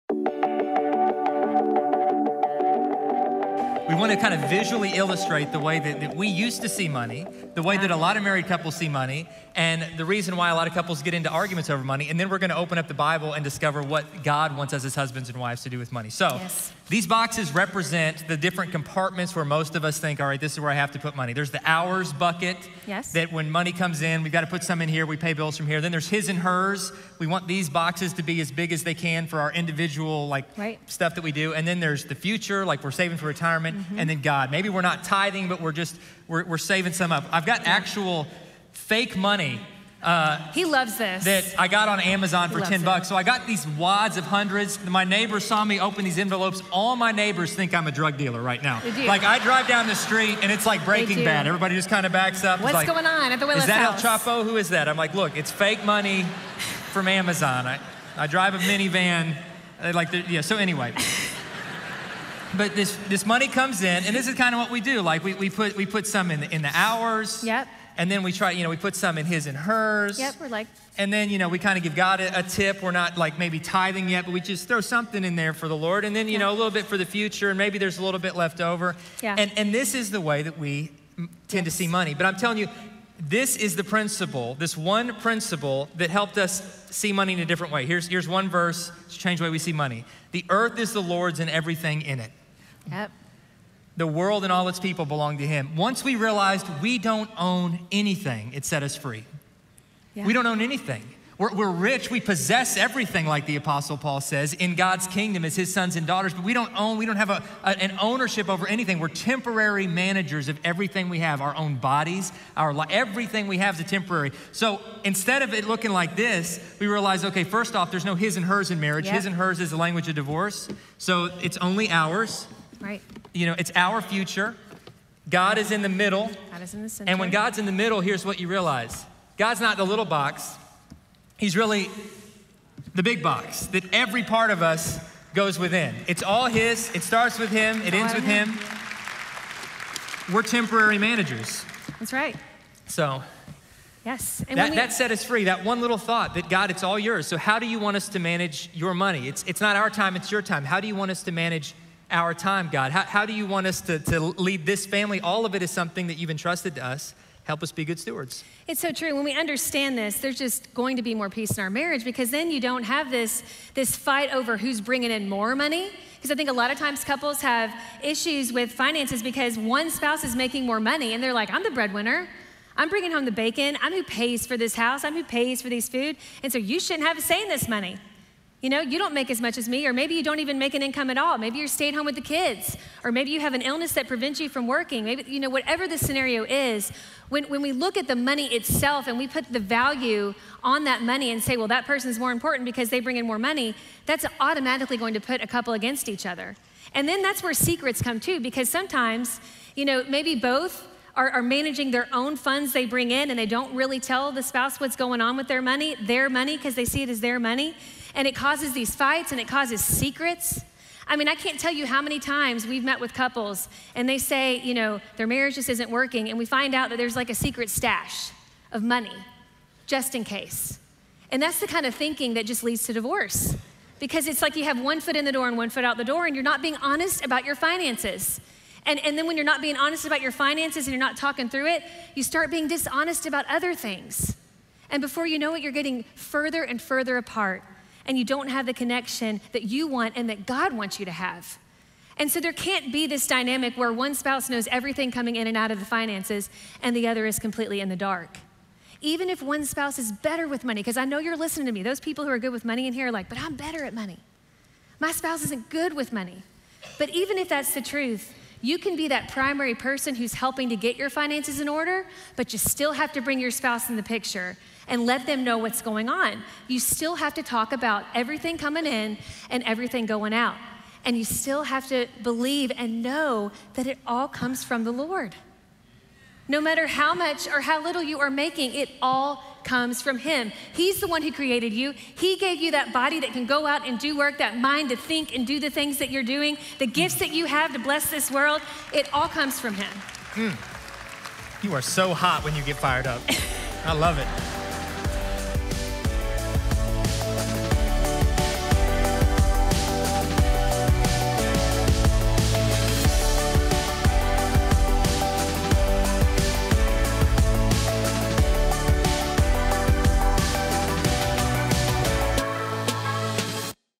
God, Marriage, and Money | Dave and Ashley Willis
The way you see money can be purposeful and peaceful. Dave & Ashley explain how to bring unity in your finances within marriage.

More marriage building content can be found at xomarriage.com